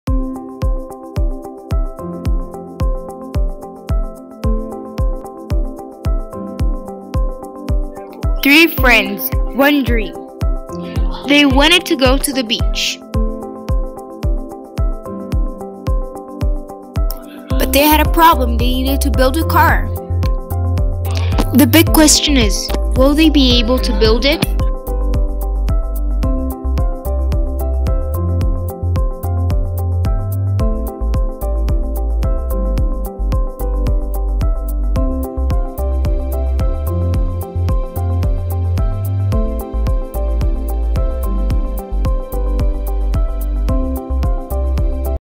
Friends trailer
Grade 8 Multimedia students made 1 minute animation movies.